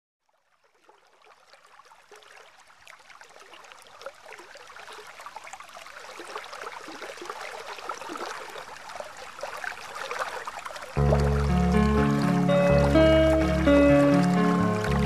water falling through a tree trunk... natural sound
water falling in the form of streams is really a beautiful natural panorama.